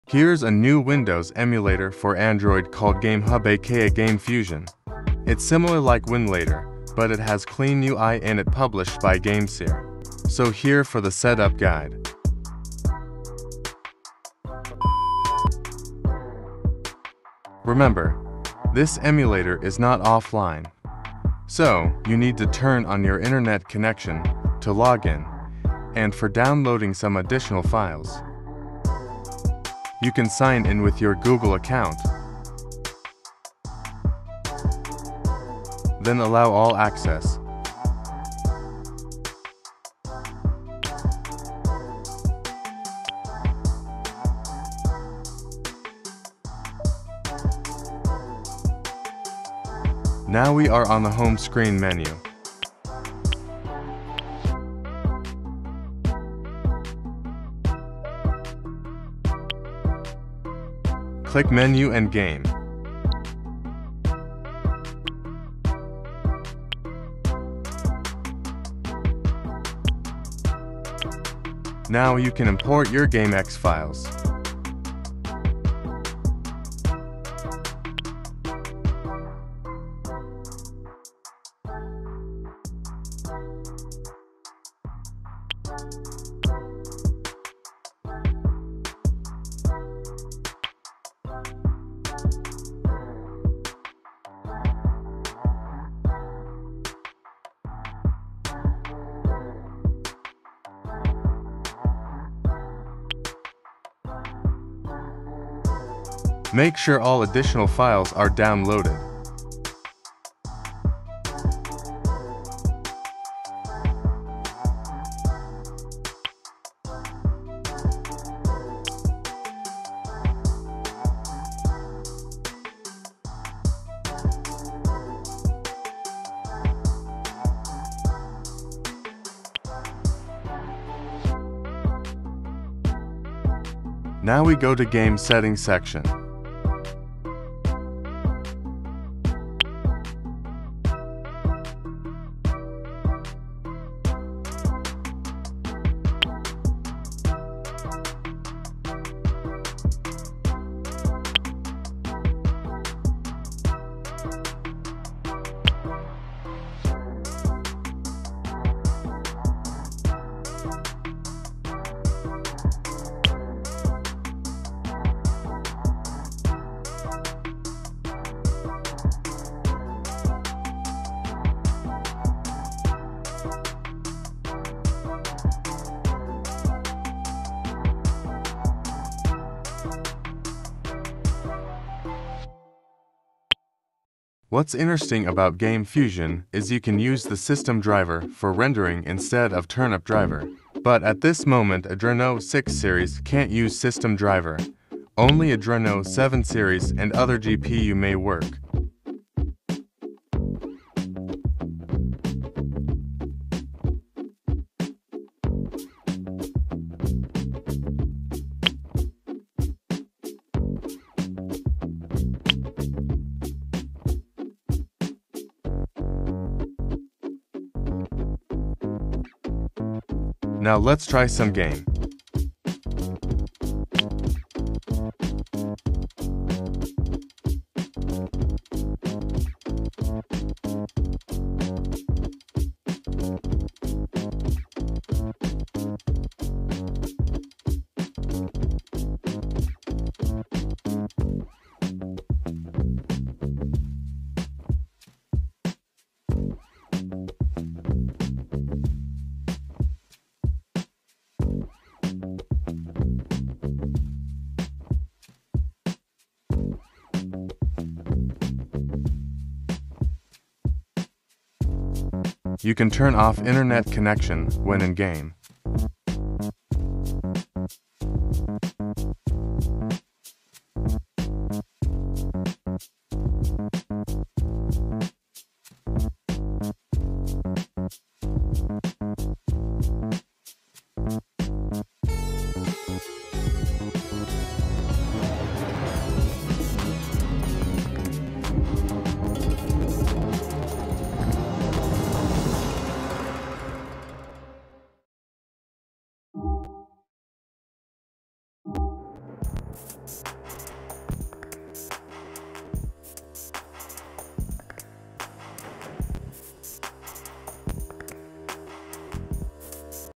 Play PC games on Android Offline! New Windows Emulator for Android - Game Fusion Setup + Settings!
New windows emulator for Android Game Fusion

📱POCO F1
- 6/128
- Snapdragon 845
- Android 13
- Arrow OS - NGK Kernel 4.3.1

 🖥⚡️Emulator info
- Gamehub aka Game Fusion by Gamesir